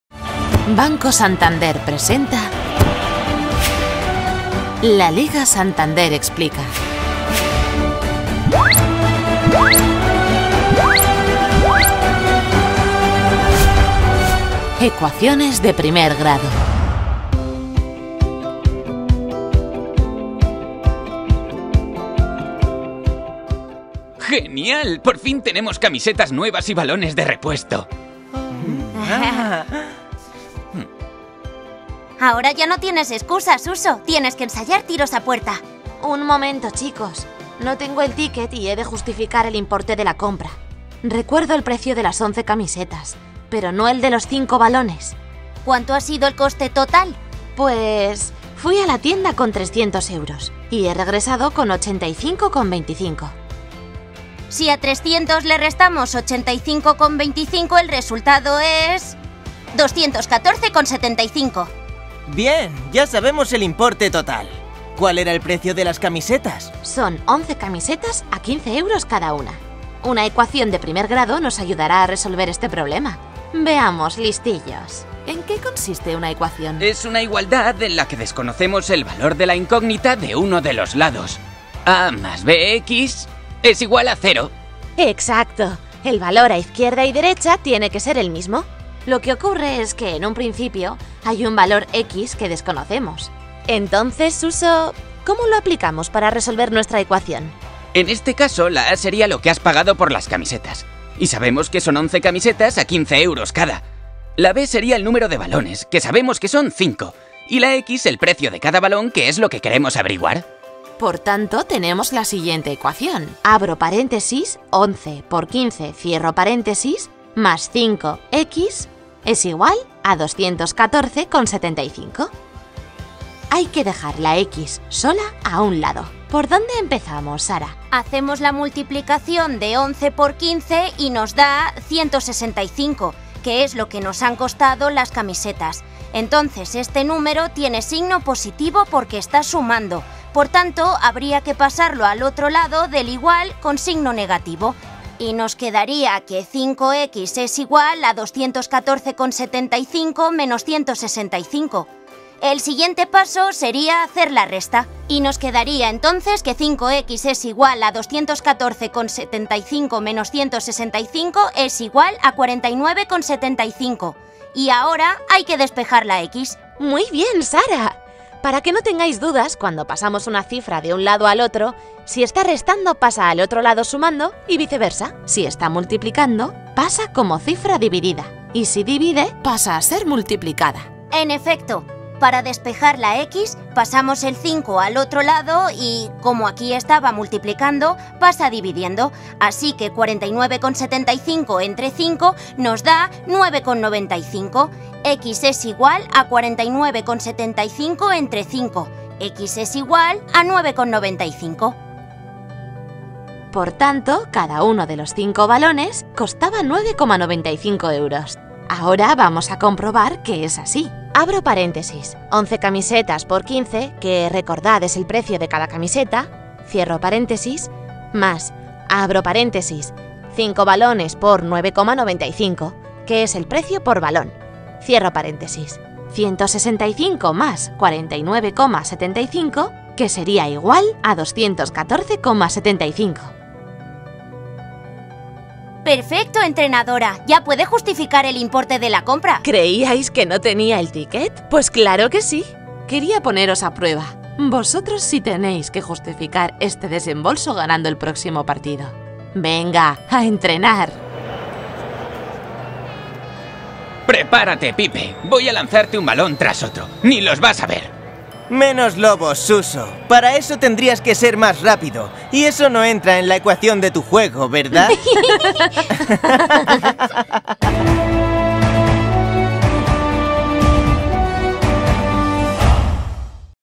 ¿Cómo hacer ecuaciones de primer grado?
¿Qué es una ecuación de primer grado? Una ecuación de primer grado es una igualdad que contiene una incógnita “x” y que no sabemos su valor. El valor de ambos lados debe ser el mismo. Para resolver la ecuación de primer grado tenemos el valor de la incógnita para que se cumpla la igualdad.

Aprende a resolver ecuaciones de primer grado con este vídeo de forma sencilla y divertida.